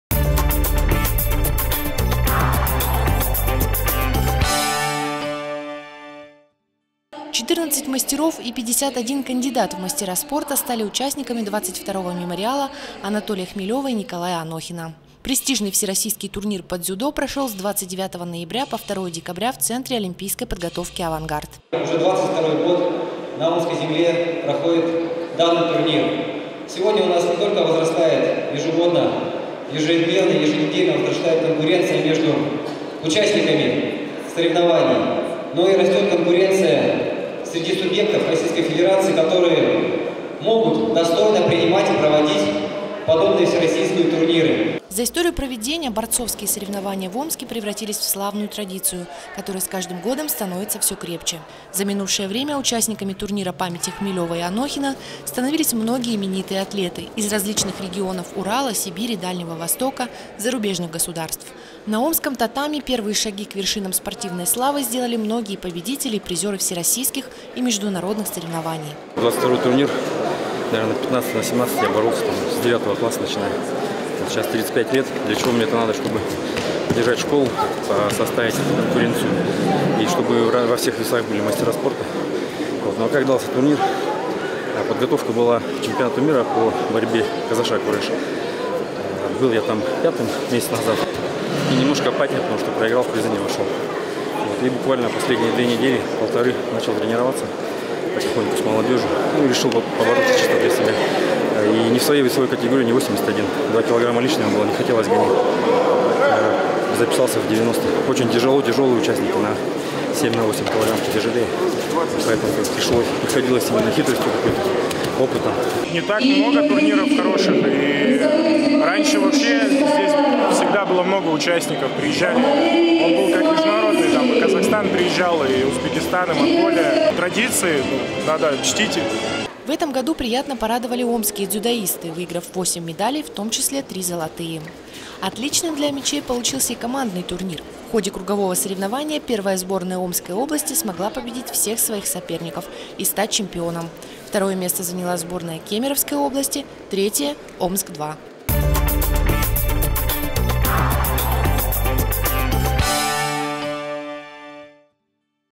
XXII всероссийский турнир по дзюдо памяти А.А. Хмелёва и Н.И. Анохина
Омские дзюдоисты завоевали восемь наград на домашнем мемориале заслуженного тренера СССР А.А. Хмелёва и мастера спорта СССР Н.И. Анохина